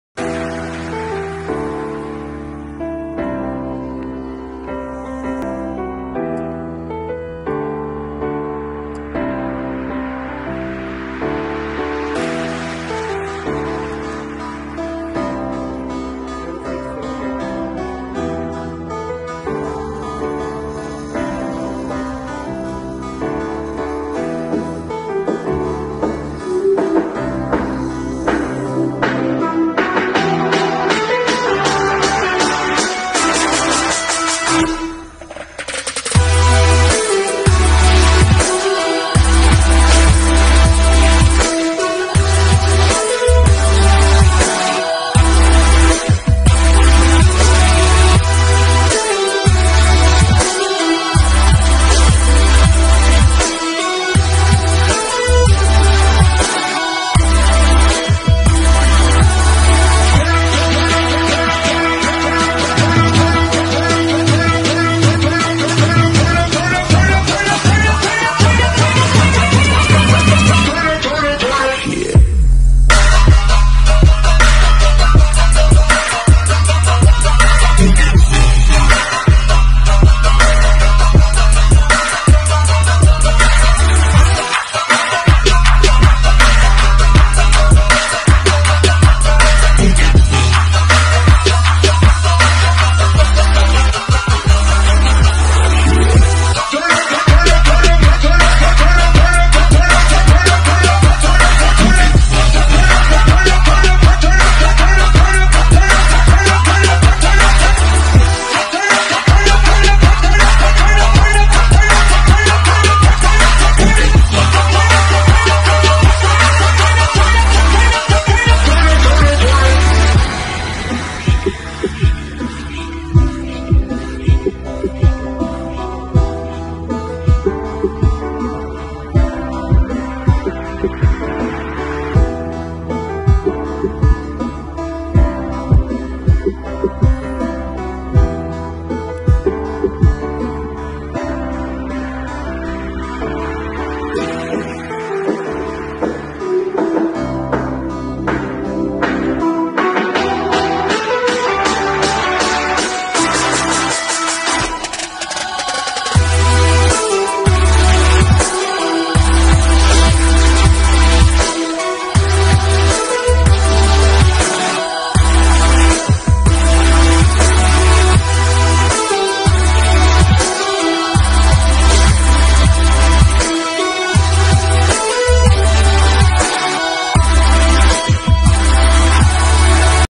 RWD RC DRIFT ADDICT IN UNITED ARAB EMIRATES, ABU DHABI , DUBAI, THODZ RC BODY WORKX, ADRCDT
Rc drifting is fun, rwd for better drift sideways.